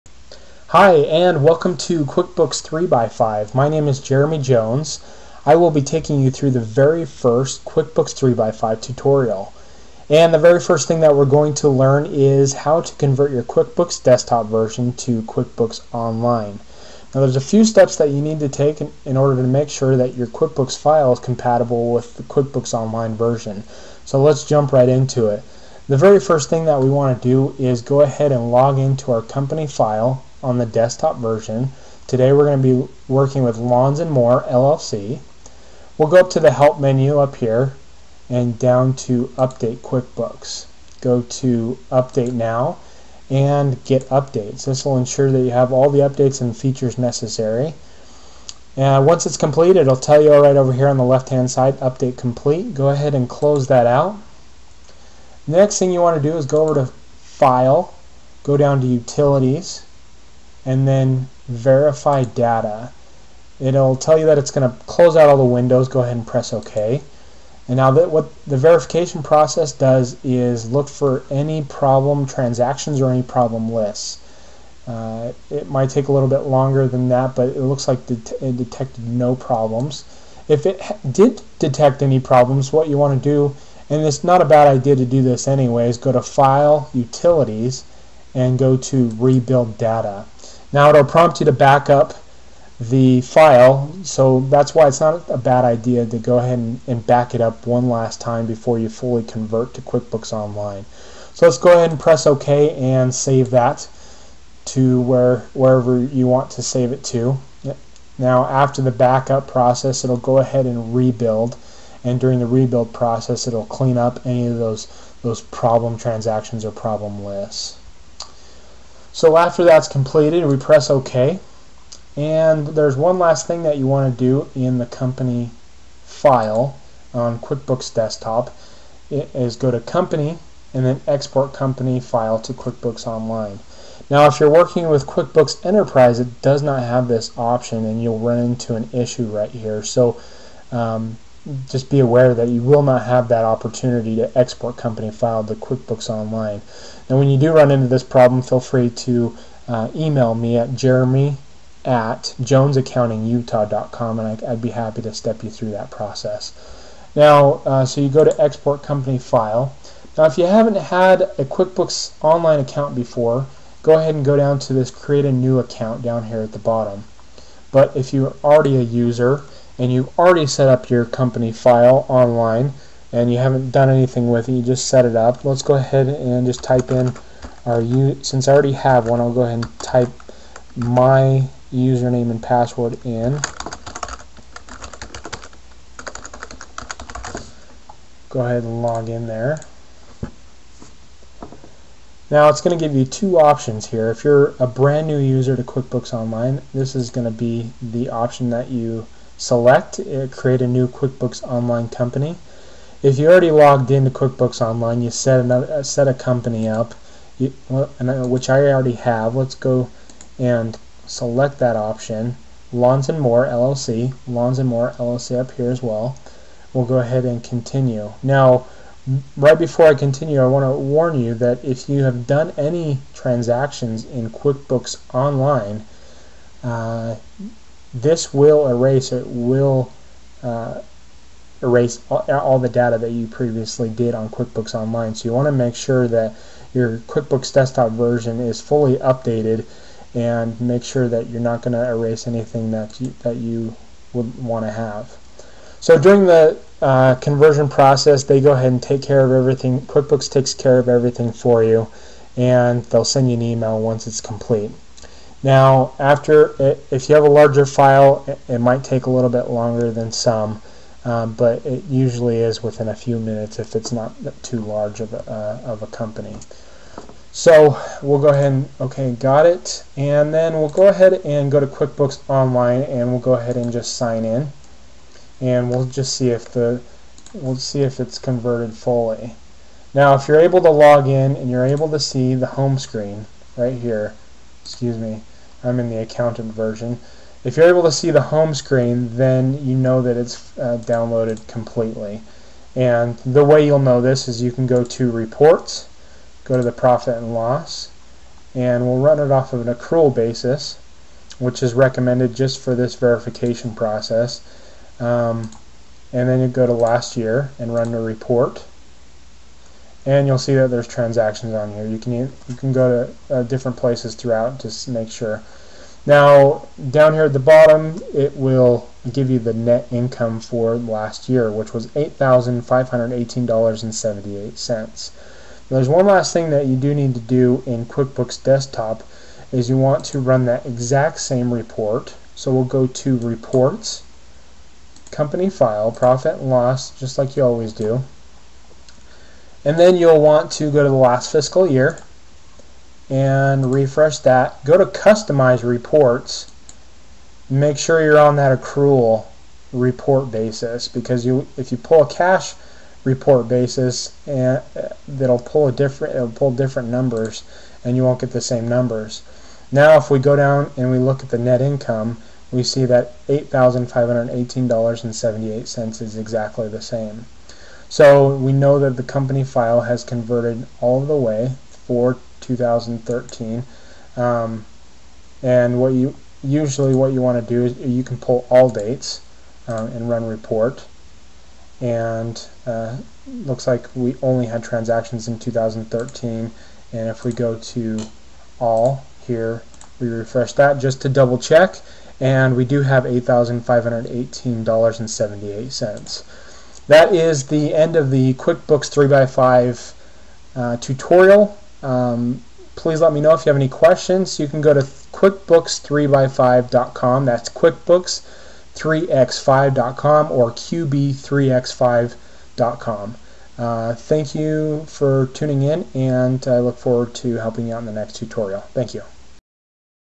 Convert QuickBooks Desktop to QuickBooks Online
How to move your QB's Desktop company file to QB's Online. qb3x5.com